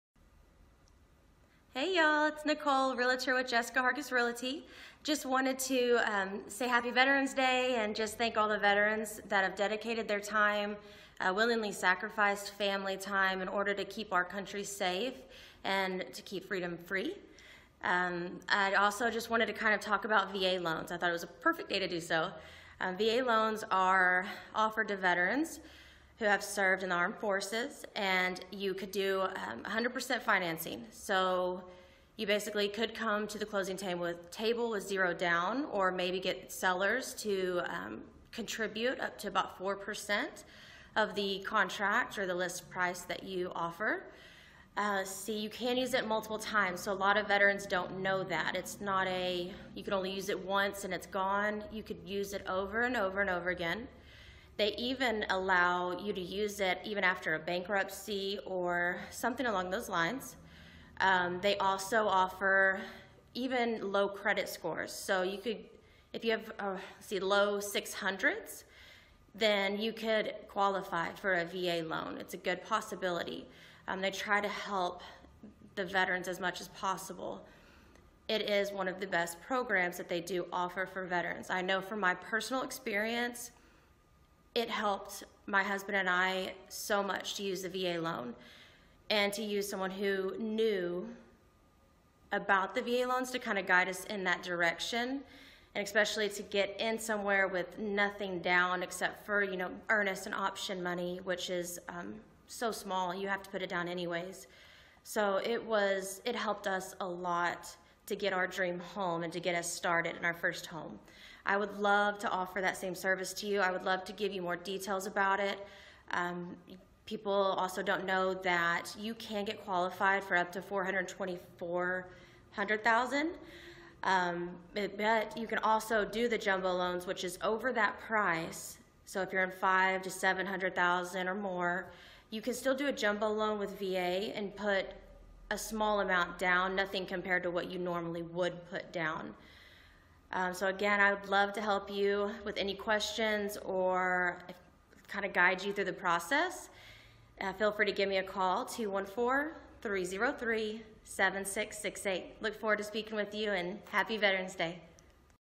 Veterans Day
Happy Veterans - explain details on VA loans